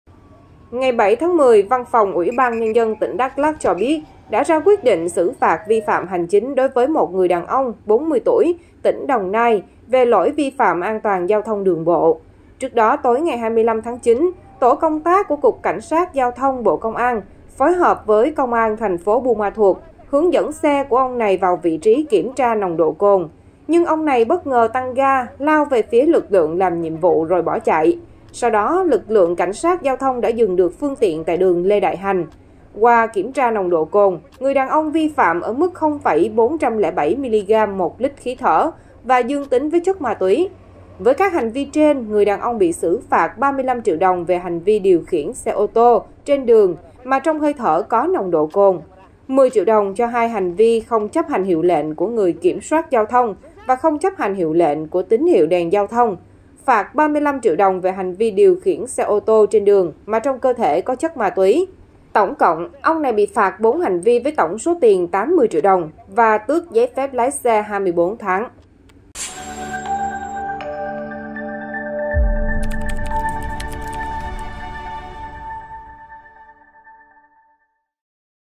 Tài xế dương tính với ma túy “thông chốt” và cảnh truy đuổi của CSGT
Ngày 7-10, Văn phòng UBND tỉnh Đắk Lắk cho biết, đã ra quyết định xử phạt vi phạm hành chính đối với một người đàn ông (40 tuổi, tỉnh Đồng Nai) về lỗi vi phạm an toàn giao thông đường bộ.